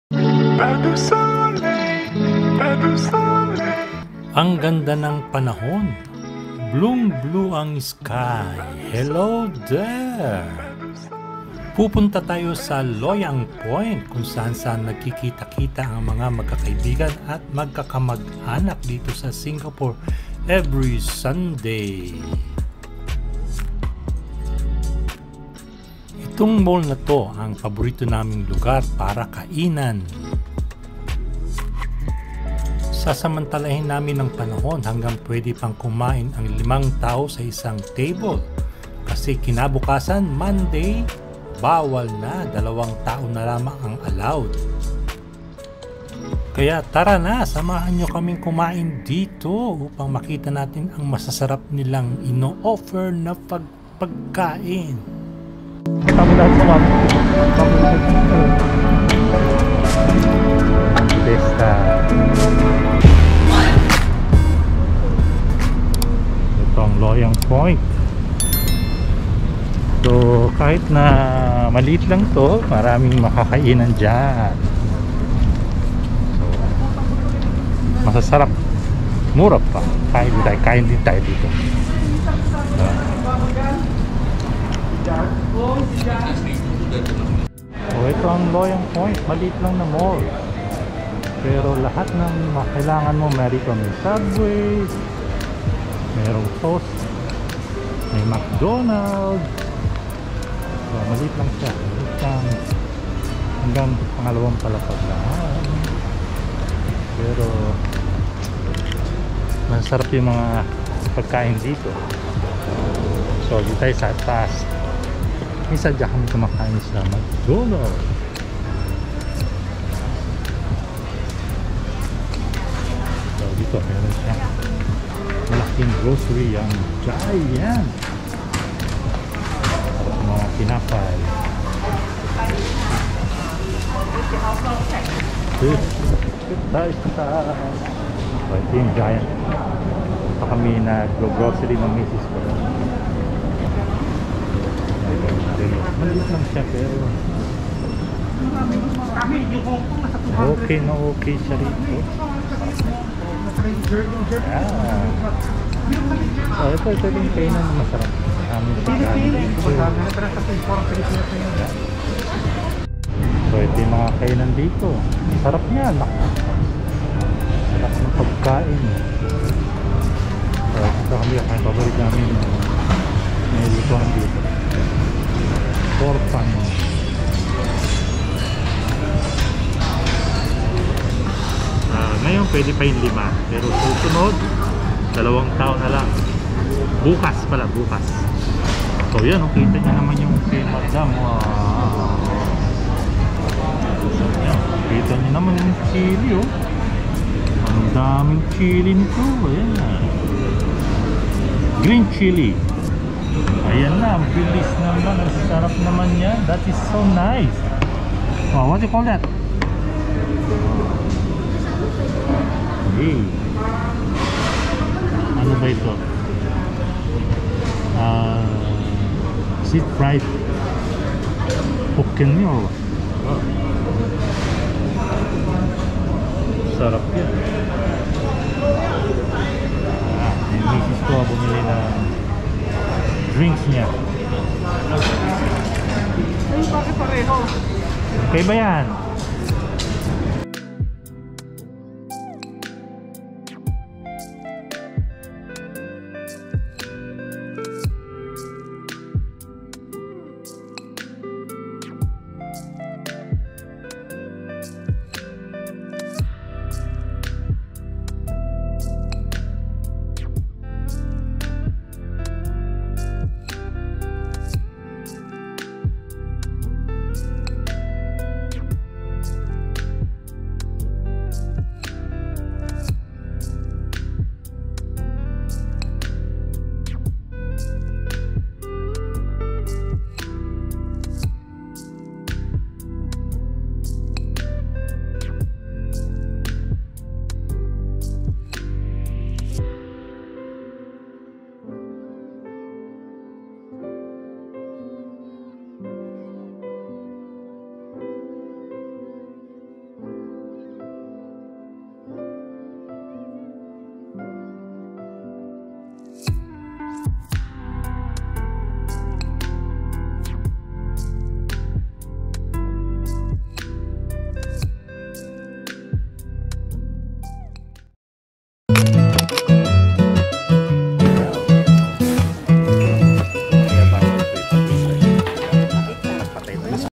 LOYANG POINT SMALL MALL BUT ALMOST YOU NEED IS HERE AND THEIR FOOD ARE GOOD AND DELICIOUS COZY TOO
Loyang Point is one of my Favorite place to hangout with friends and eat, we are taking advantage of this time to eat together with friends of five because tomorrow which is Monday five people are no longer allowed to eat together but by two person only will be allowed.